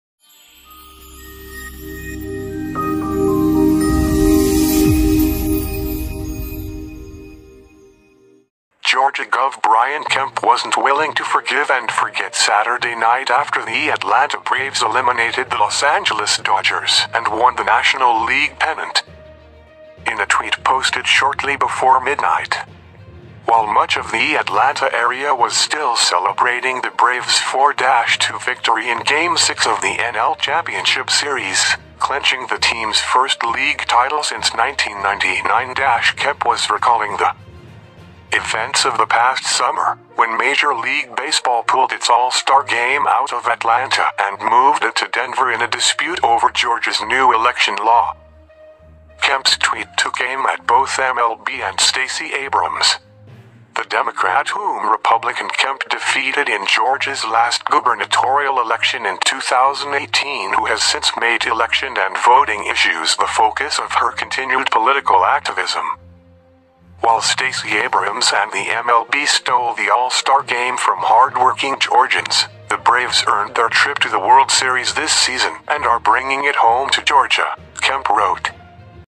Braves pennant win prompts GOP's Brian Kemp to jab at Stacey Abrams, MLB over Atlanta All-Star snub
Braves pennant win prompts GOP's Brian Kemp to jab at Stacey Abrams, MLB over Atlanta All-Star snubWhile Stacey Abrams and the MLB stole the All-Star Game from hardworking Georgians, the Braves earned their trip to the World Series this season and are bringing it home to Georgia," Kemp wrote.Georgia Gov. Brian Kemp wasn’t willing to forgive and forget Saturday night after the Atlanta Braves eliminated the Los Angeles Dodgers and won the National League pennant.

In a tweet posted shortly before midnight, while much of the Atlanta area was still celebrating the Braves’ 4-2 victory in Game 6 of the NL Championship Series – clinching the team’s first league title since 1999 – Kemp was recalling the events of the past summer, when Major League Baseball pulled its All-Star Game out of Atlanta and moved it to Denver in a dispute over Georgia’s new election law.

Kemp’s tweet took aim at both MLB and Stacey Abrams, the Democrat whom Republican Kemp defeated in Georgia’s last gubernatorial election in 2018 who has since made election and voting issues the focus of her continued political activism.